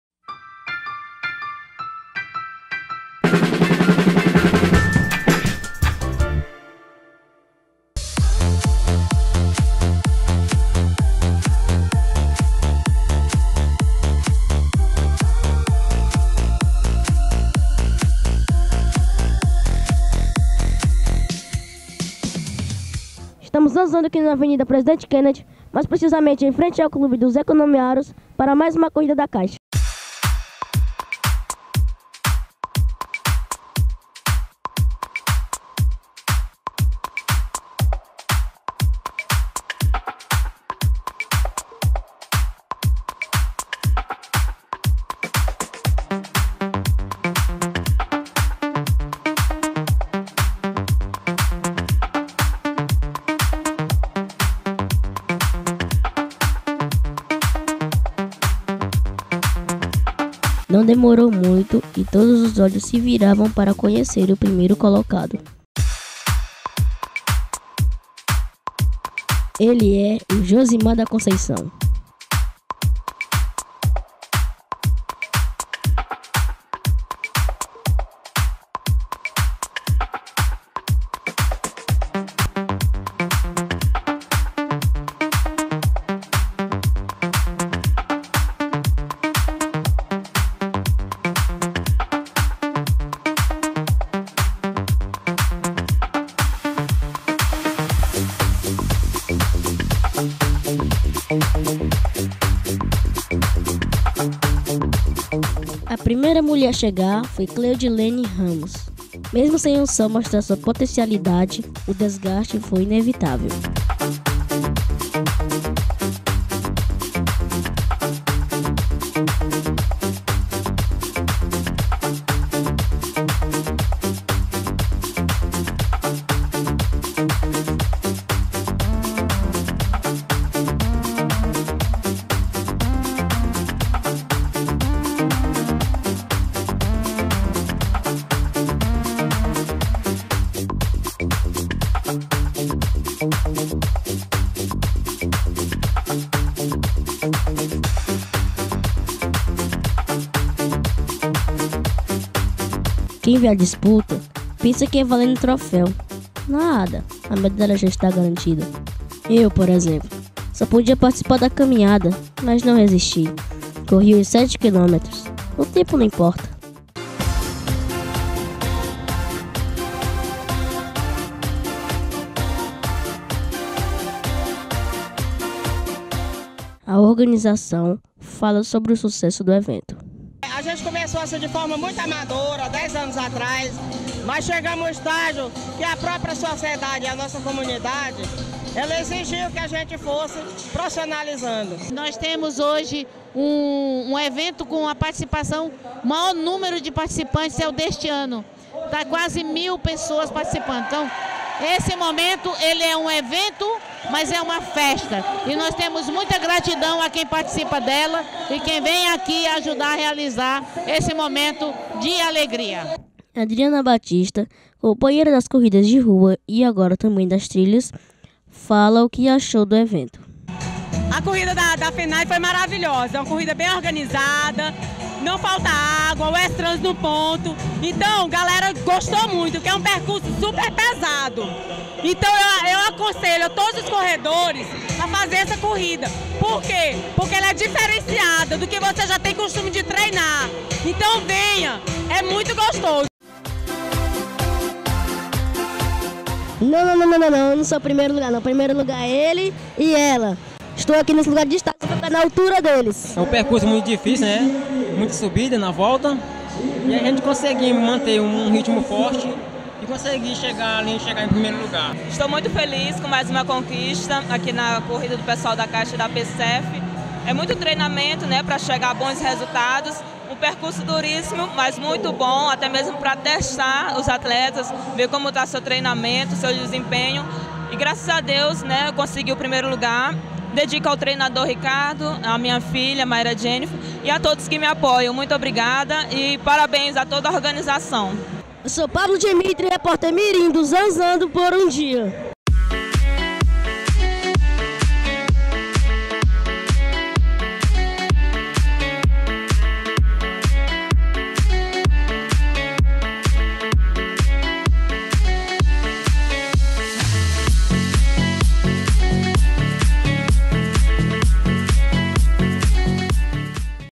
Zanzando: Corrida Fenae do Pessoal da Caixa 2018
Não pudemos zanzar na corrida da Caixa realizada no último domingo mas enviamos um repórter especial. Dimitri é fã de vários tipos de esportes, um deles, a corrida.